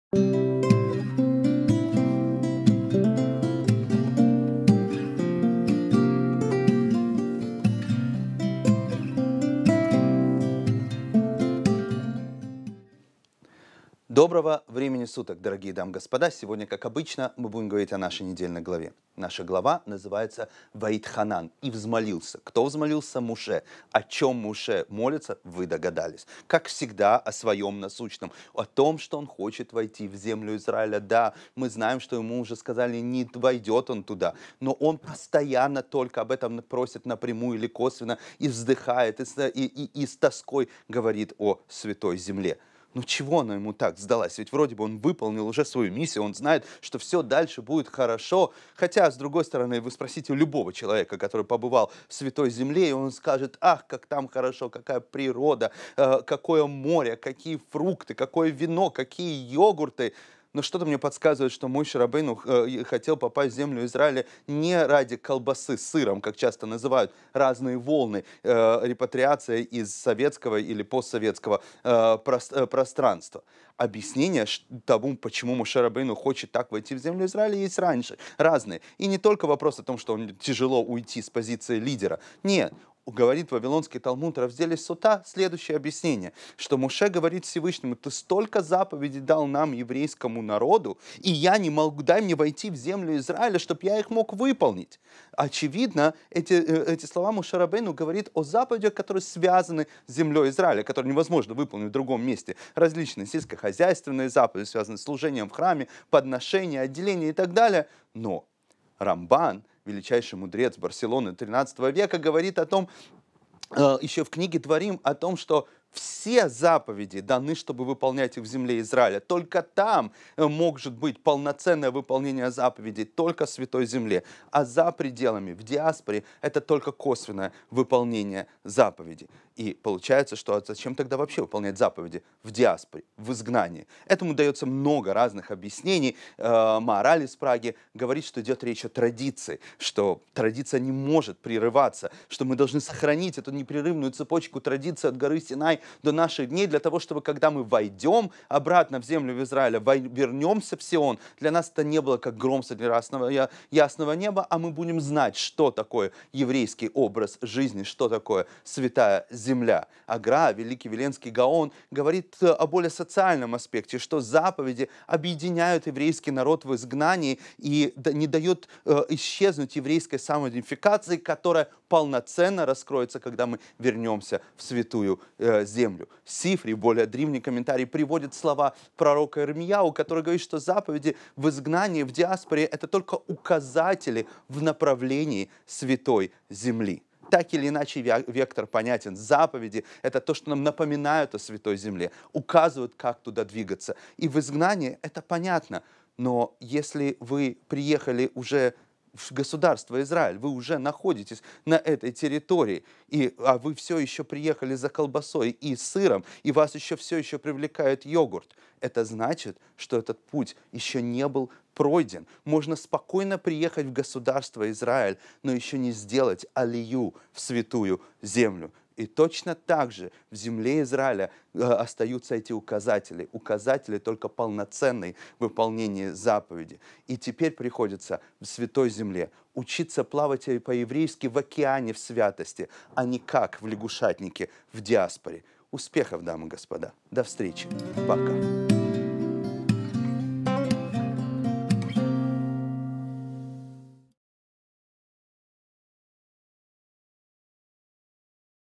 ⬆ Ваэтханан 5781. Указатели | Недельная глава с равом Альтшулем
Доброго времени суток, дорогие дамы и господа! Сегодня, как обычно, мы будем говорить о нашей недельной главе.
Наша глава называется Ваэтханан - "И взмолился". Кто взмолился? Моше. О чем Моше молится - вы догадались. Как всегда, о своем насущном. О том, что он хочет войти в землю Израиля.

Да, мы знаем, что ему уже сказали - не войдет он туда. Но он постоянно только об этом и просит - и напрямую, и косвенно, и вздыхает, и с тоской говорит о Святой земле...